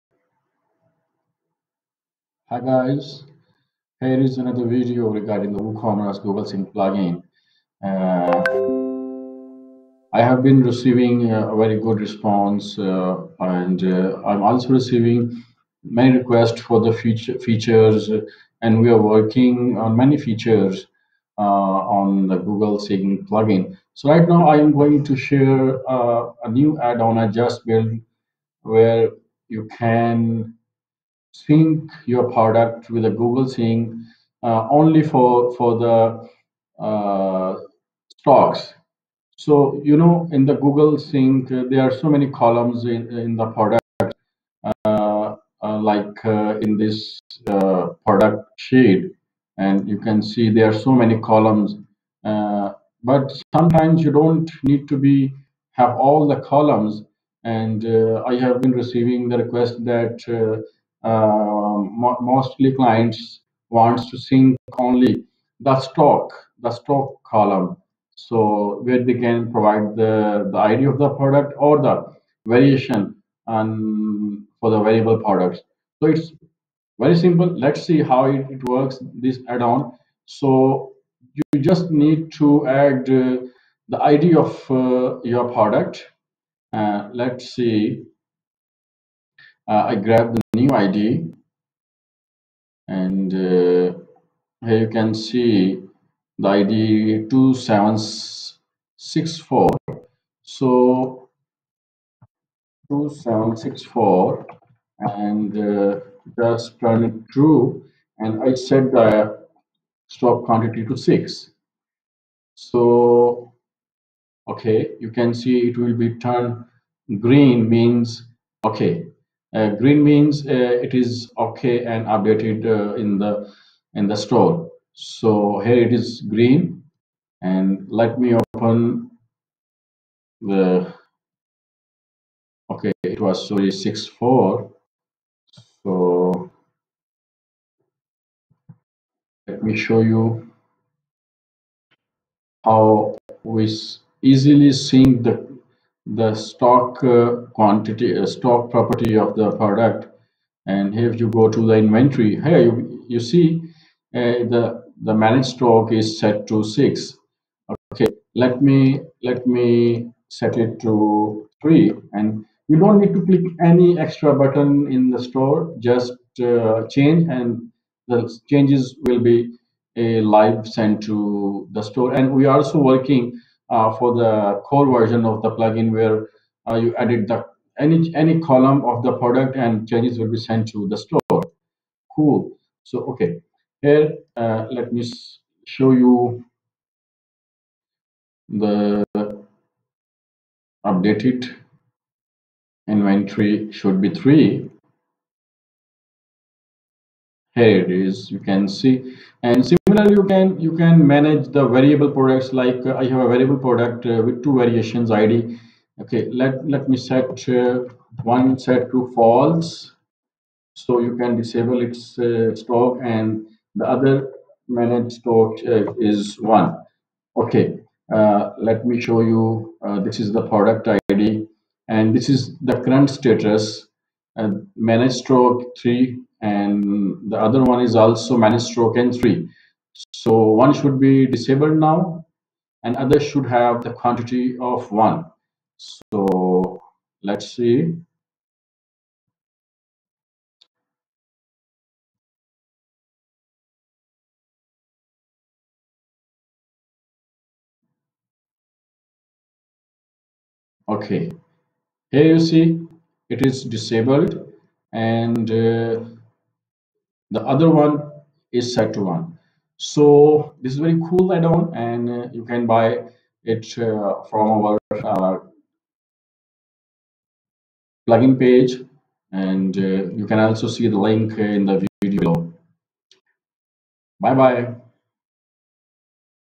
Sync Stock with WooCommerce Stock Addon
Sync only your product stock/quantities with Google Sync Addon. No extra columns. Live sync quickly updates product or variation quantity back into the store.